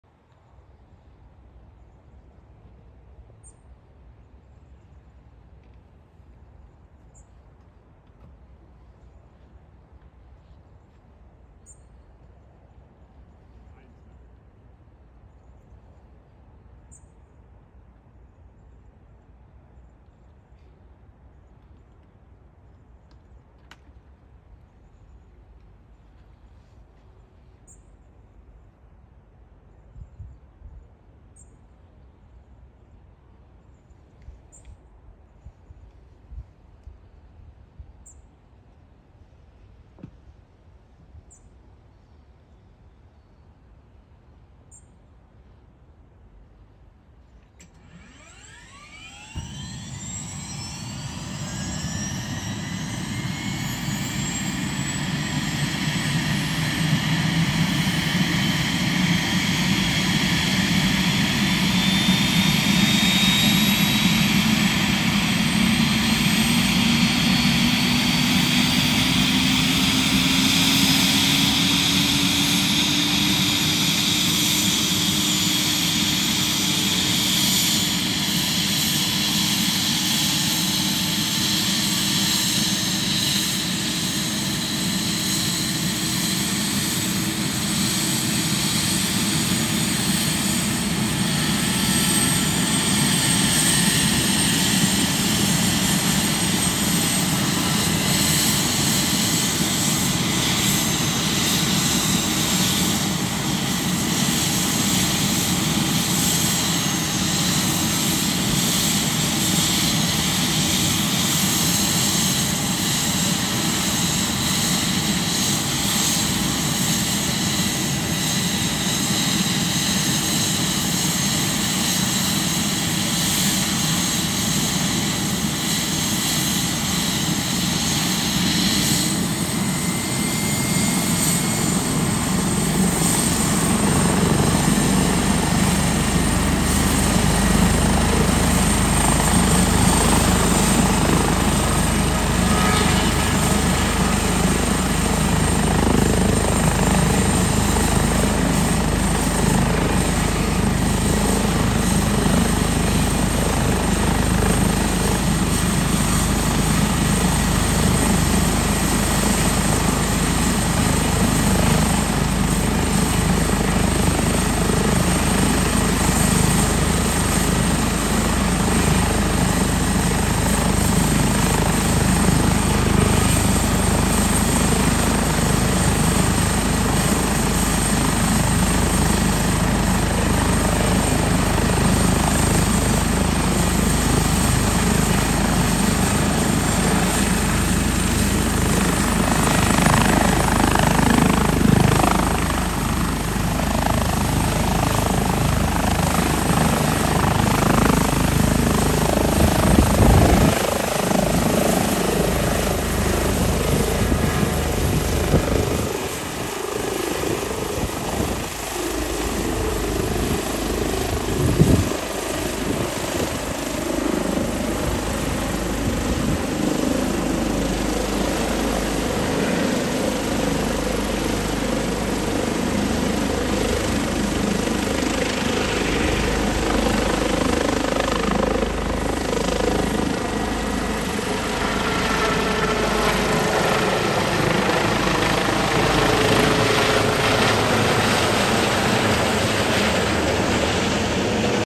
DRF Luftrettung | Start Christoph Weser / 55 | Airbus H145 | D-HDSK | Klinikum Wilhelmshaven
Christoph Weser startet am Klinikum Wilhelmshaven mit Patient in Richtung Oldenburg.